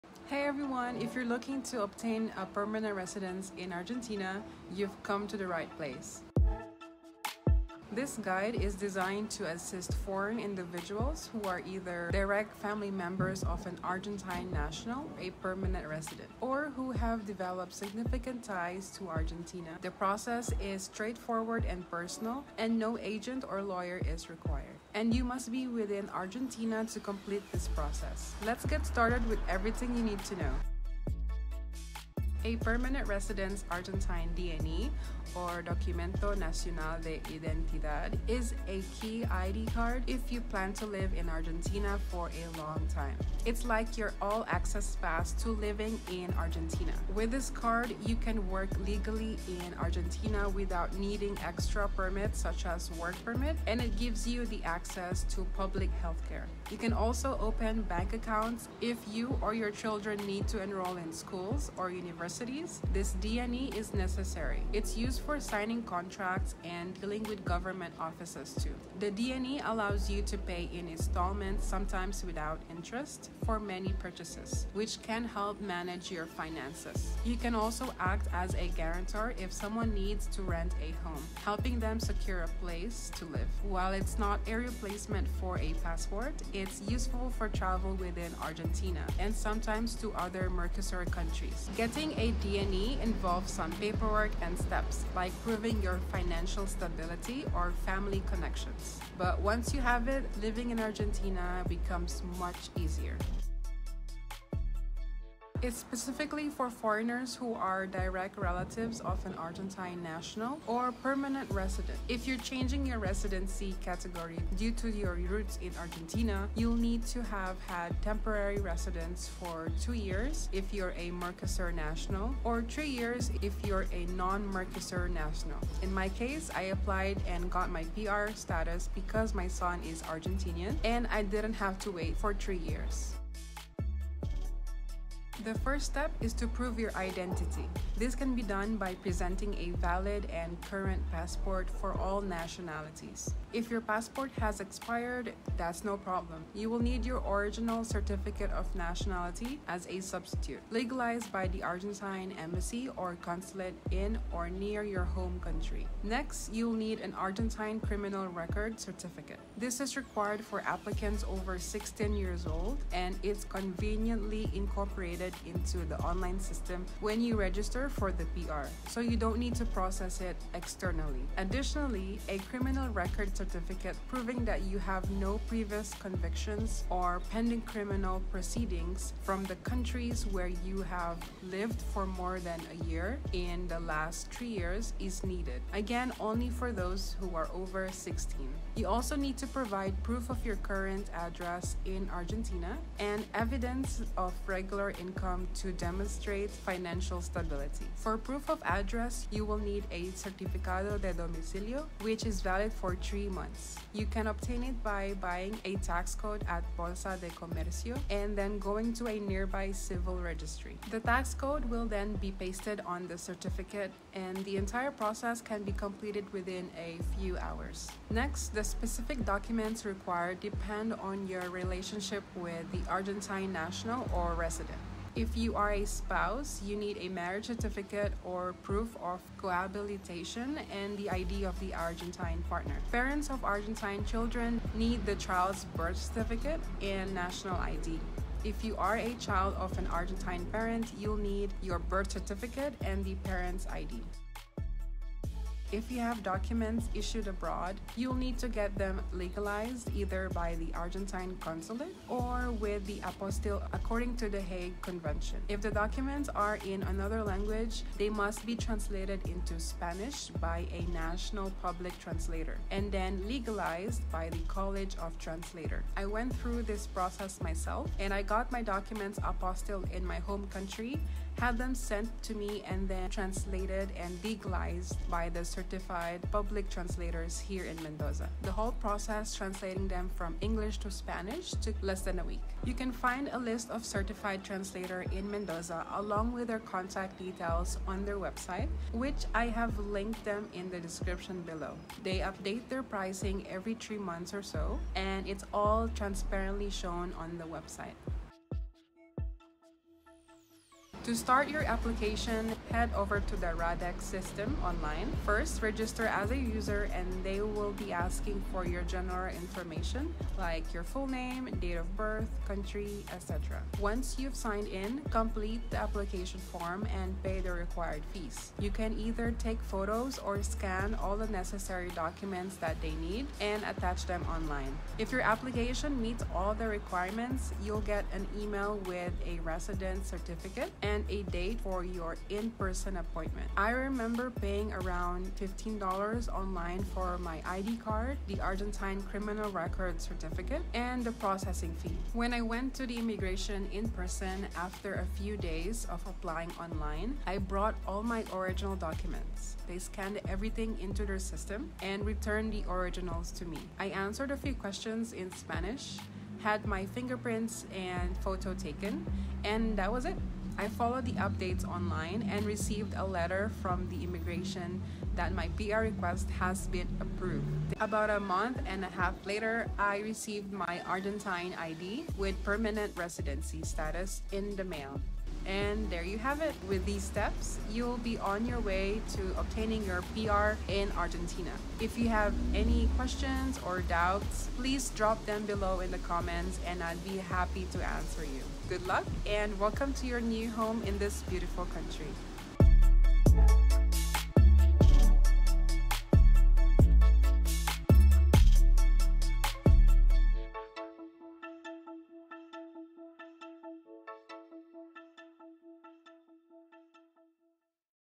How to Become a Permanent Resident in Argentina (Step by Step Guide)
Want to get permanent residency in Argentina?
This video breaks down the entire process for you.

Video Breakdown
0:00 - Introduction to obtaining permanent residence in Argentina
0:31 - What is the Argentine DNI (Documento Nacional)?
1:44 - Eligibility criteria for permanent residence
2:14 - Steps to prove your identity and documents required
4:05 - Take note
5:00 - Starting your application through the Radex system
6:02 - Final steps and receiving your Argentine ID